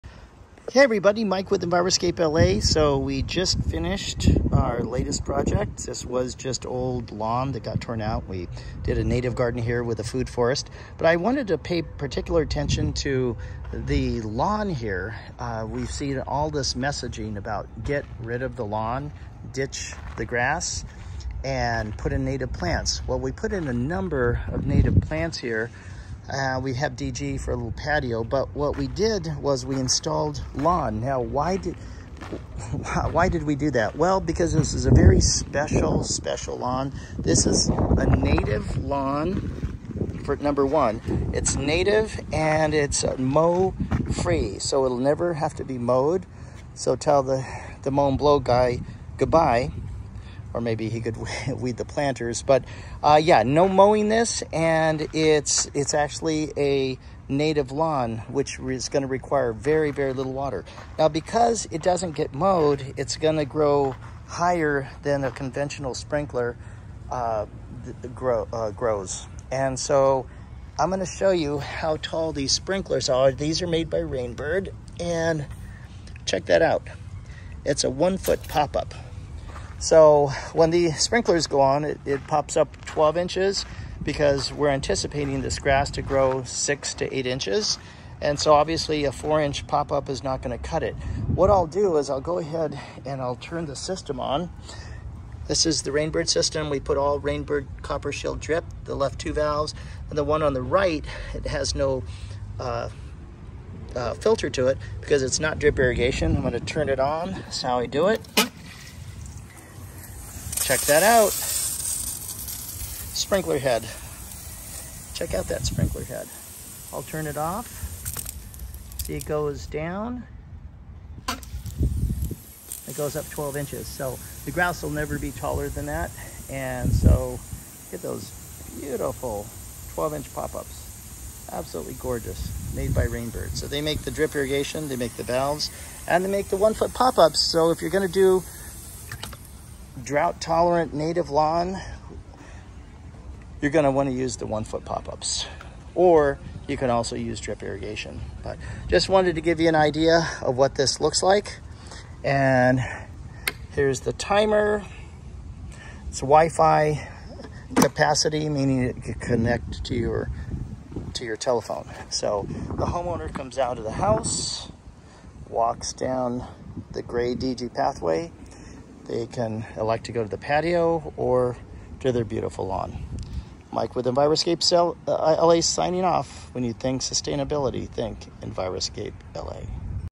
Native mow free lawn!
Yep, it’s real!  Native lawn which doesn’t need to be mowed really exists. It requires very little water once established.